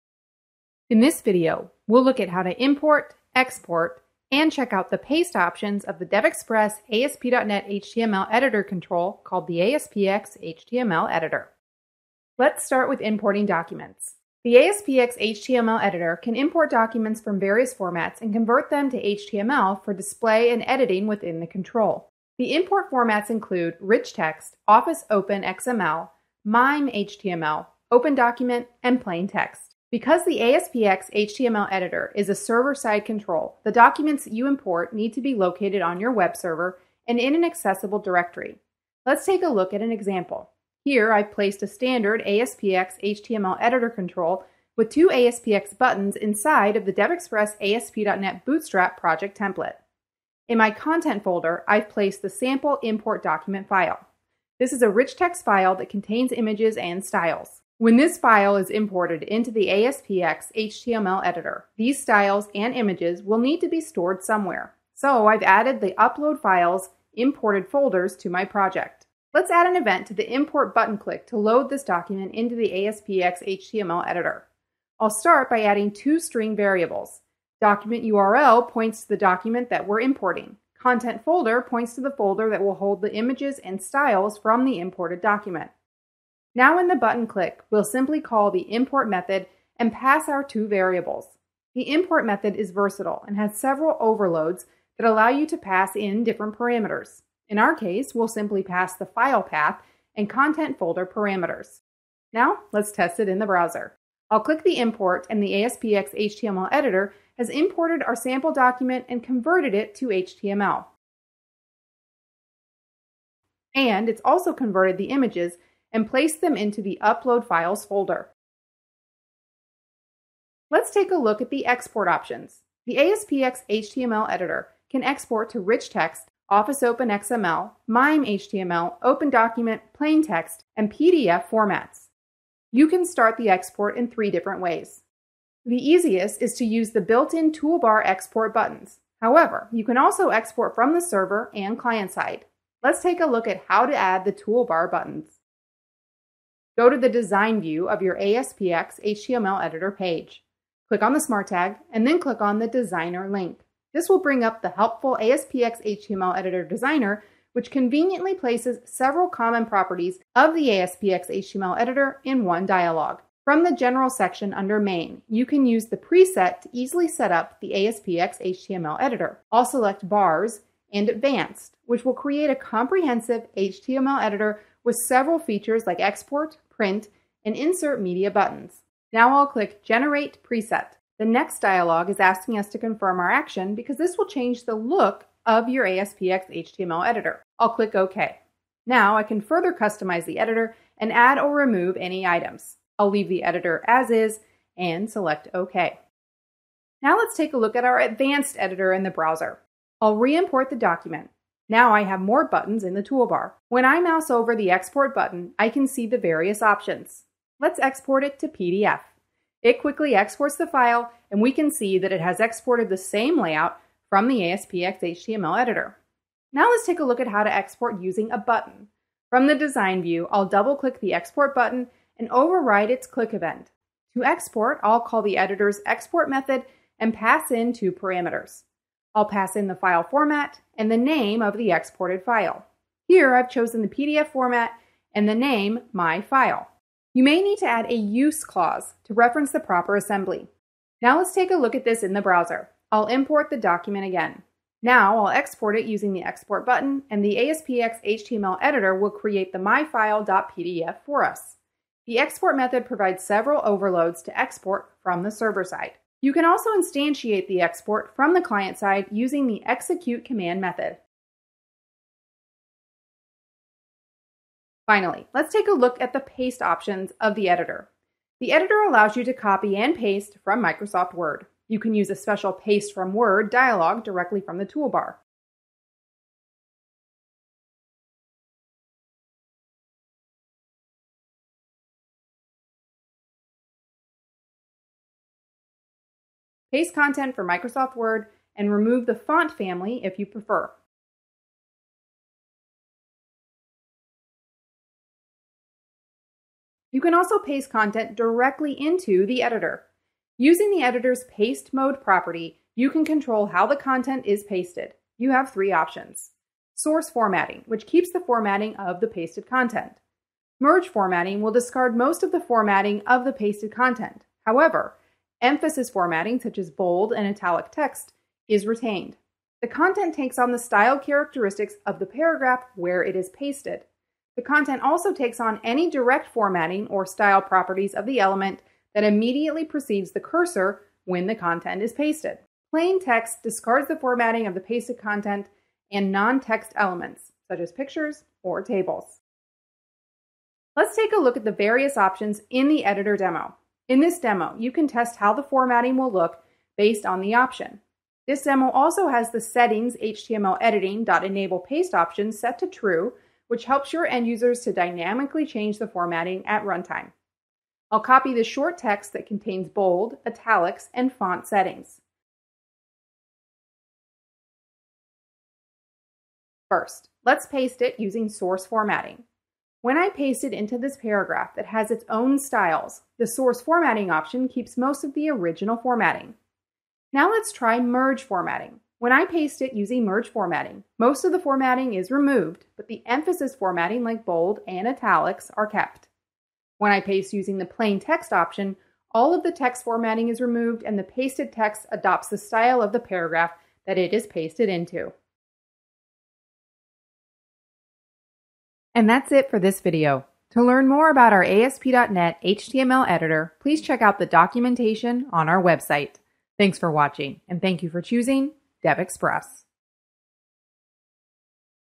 ASP.NET HTML Editor - Import, Export, and Pasting
Learn about the Import, export, and pasting features for the DevExpress ASP.NET HTMLEditor.